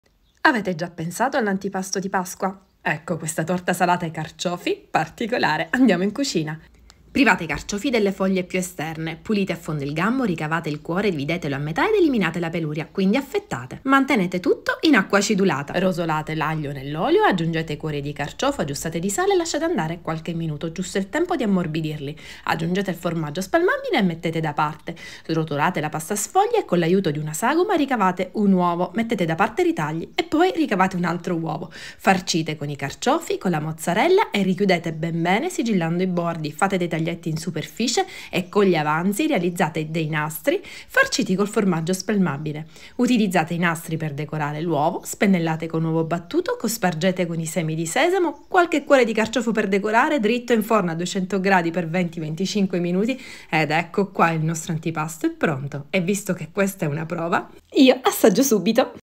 Uovo di Pasqua salato con i carciofi
Ingredienti per fare l’Uovo di Pasqua salato con i carciofi
2 rotoli pasta sfoglia (tonda o rettangolare)
6 cuori di carciofo (300g circa)
100 g formaggio spalmabile
q.b. sale
1 spicchio aglio
q.b. uovo (battuto per la finitura)
100 g mozzarella per pizza (o provola)
q.b. pepe nero
q.b. semi di sesamo